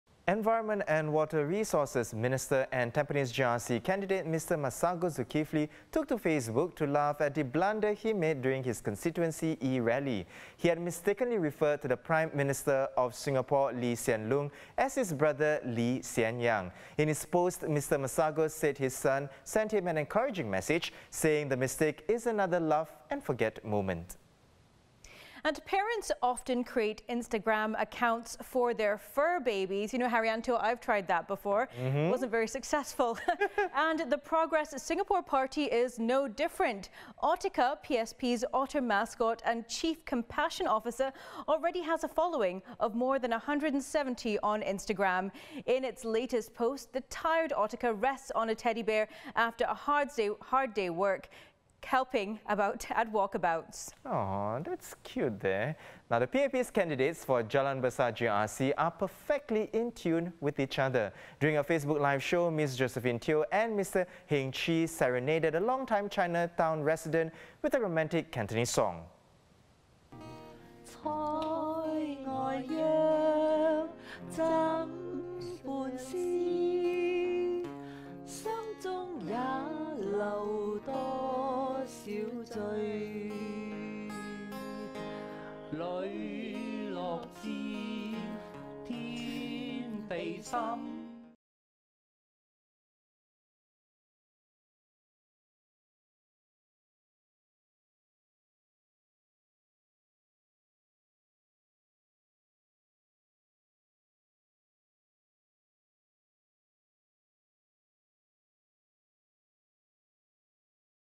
Social media highlights | GE2020 TODAY
Environment and Water Resources Minister and Tampines GRC candidate Mr Masagos Zulkifli took to Facebook to laugh at the blunder he made during his constituency e-rally.

He had mistakenly referred to the Prime Minister of Singapore, Lee Hsien Loong, as his brother Lee Hsien Yang.

Meanwhile, Progress Singapore Party (PSP) created an Instagram account of its cute otter mascot, Otica.

And People Action's Party (PAP) Jalan Besar GRC candidates Ms Josephine Teo and Mr Heng Chee How serenaded a resident with a romantic Cantonese song.